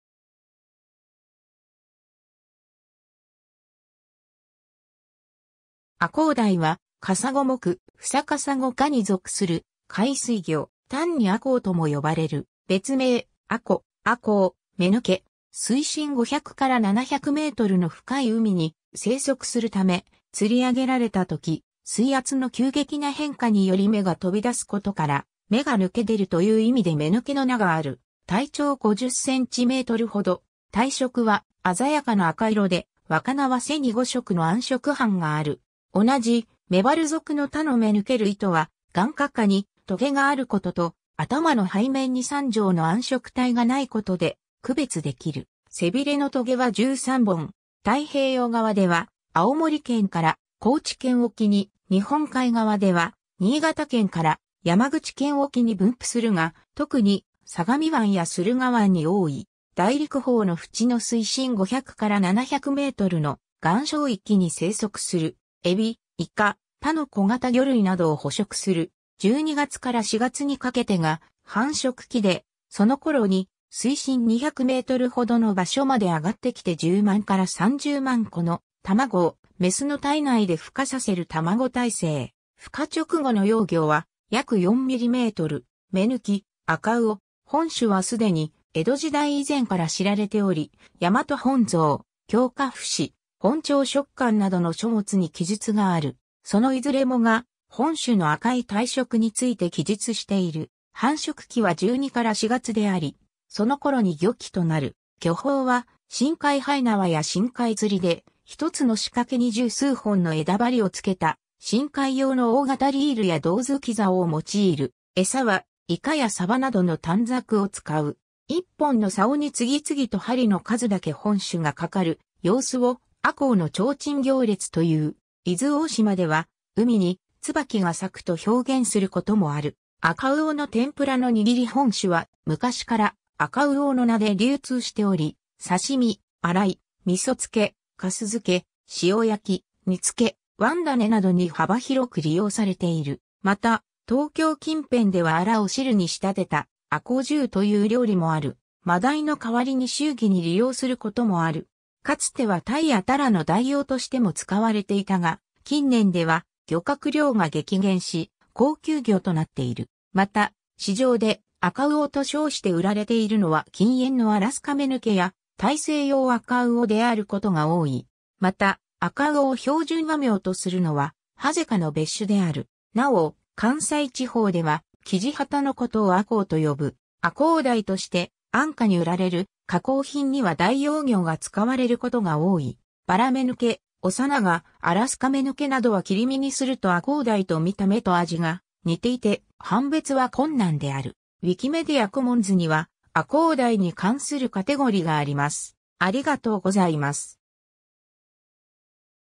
アコウダイ
アコウダイ（赤魚鯛、学名：Sebastes matsubarae）はカサゴ目フサカサゴ科（あるいはメバル科）に属する海水魚。
単にアコウとも呼ばれる。
別名、アコ、アコウ、メヌケ（他のメヌケ類との混称として使われる）。
水深500-700mの深い海に生息するため、釣り上げられたとき、水圧の急激な変化により目が飛び出すことから、「目が抜け出る」という意味でメヌケの名がある。
 体長50cmほど。
体色は鮮やかな赤色で、若魚は背に5色の暗色斑がある。
同じメバル属の他のメヌケ類とは、眼窩下に棘があることと、頭の背面に3条の暗色帯が無いことで区別できる。
背びれの棘は13本。
 太平洋側では青森県から高知県沖に、日本海側では新潟県から山口県沖に分布するが、特に相模湾や駿河湾に多い。
 大陸棚の淵の水深500-700mの岩礁域に生息する。
エビ、イカ、他の小型魚類などを捕食する。
 12月から4月にかけてが繁殖期で、そのころに水深200mほどの場所まで上がってきて10万-30万個の卵をメスの体内で孵化させる卵胎生。
孵化直後の幼魚は約4mm。
 メヌキ（東京市場）、アカウオ（富山など）。
 本種は既に江戸時代以前から知られており、大和本草、興化府志、本朝食鑑などの書物に記述がある。
そのいずれもが、本種の赤い体色について記述している。
 繁殖期は12-4月であり、そのころに漁期となる。
漁法は深海延縄や深海釣りで、一つの仕掛けに10数本の枝針を付けた深海用の大型リールや胴突き竿を用いる。
 餌はイカやサバなどの短冊を使う。
 一本の竿に次々と針の数だけ本種がかかる様子を『アコウのちょうちん行列』という。
伊豆大島では『海に椿が咲く』と表現することもある。
 赤魚の天ぷらのにぎり 本種は昔から赤魚の名で流通しており、刺身、洗い、味噌漬、粕漬、塩焼、煮付け、椀だねなどに幅広く利用されている。
 また、東京近辺ではあらを汁に仕立てた『あこう汁』という料理もある。
マダイの代わりに祝儀に利用することもある。
 かつては鯛や鱈の代用としても使われていたが、近年では漁獲量が激減し、高級魚となっている。
また、市場で「赤魚」と称して売られているのは近縁のアラスカメヌケやタイセイヨウアカウオであることが多い。
また、アカウオを標準和名とするのはハゼ科の別種である。
なお、関西地方ではキジハタのことをアコウと呼ぶ。
 「アコウダイ」として安価に売られる加工品には代用魚が使われることが多い。
バラメヌケ、オサナガ、 アラスカメヌケなどは切り身にするとアコウダイと見た目と味が似ていて判別は困難である。
 ウィキメディア・コモンズには、アコウダイに関するカテゴリがあります。